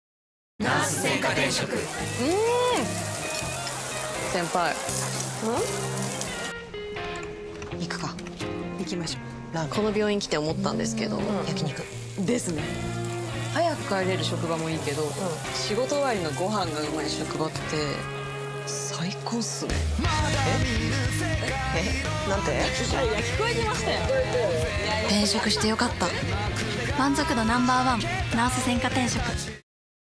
【HD】齊藤京子 CM ナース専科 転職「仕事終わりの焼肉」
齊藤京子 CM ナース専科 転職「仕事終わりの焼肉」

・CM出演
　齊藤京子、ヒコロヒー

初めての転職を経て新しい職場で働きはじめた齊藤京子演じる後輩看護師と、
転職経験豊富なヒコロヒー演じる先輩看護師が、
転職後のリアルな感情を描く。